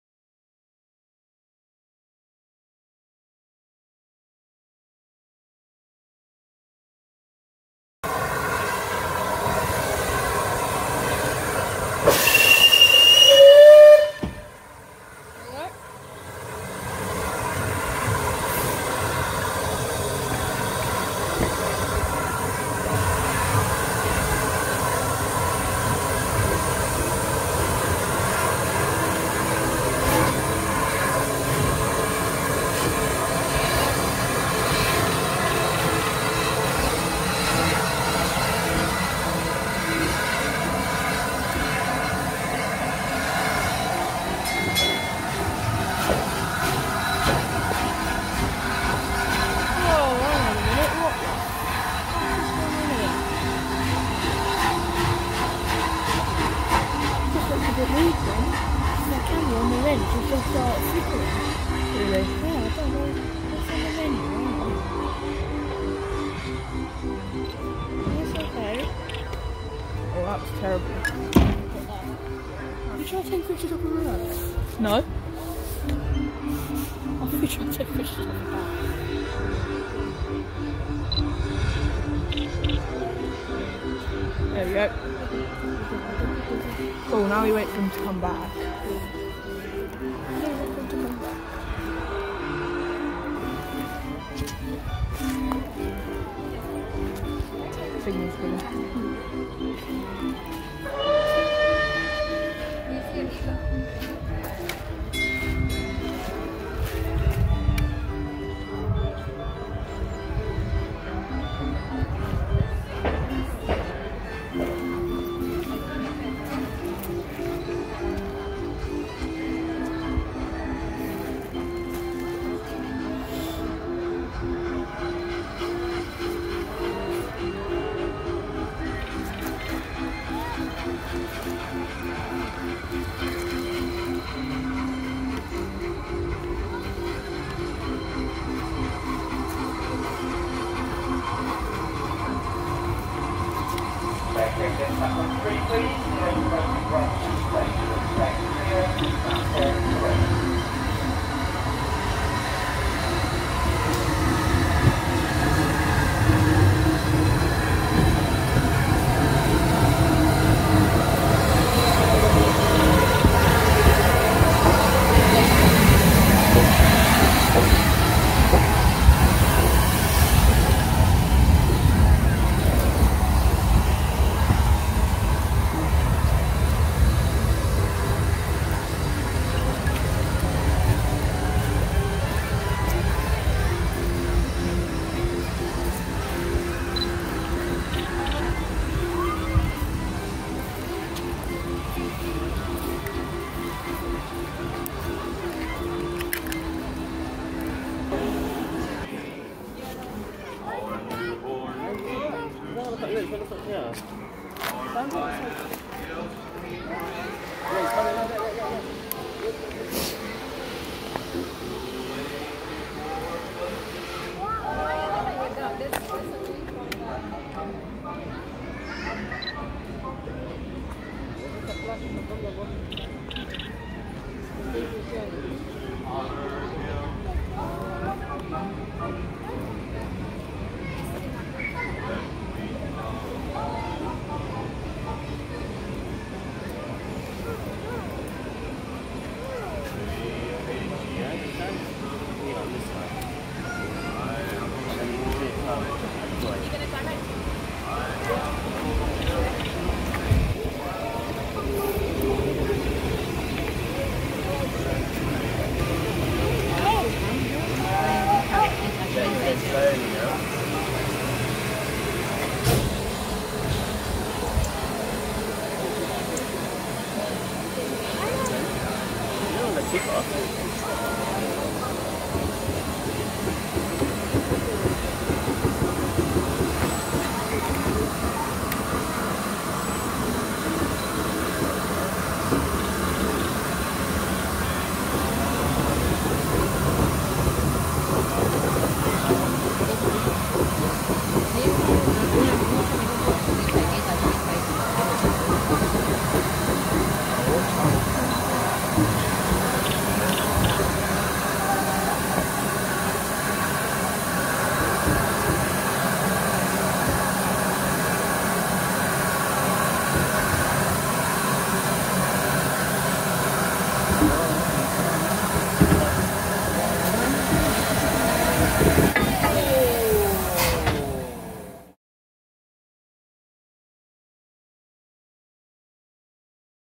The Bluebell steam Railway Santa special Unit 34059 at Horstead Keynes
Back in December 2023, Me and some of my family went to ride on the Bluebell Railway once again after quite some time, and luckily there were 2 fixtures (16/11/2023, 16/12/2023). So we decided to take the 16/12/2023 one since it is closer to Christmas but the only downside is that it was absolutely packed. I hope you enjoy.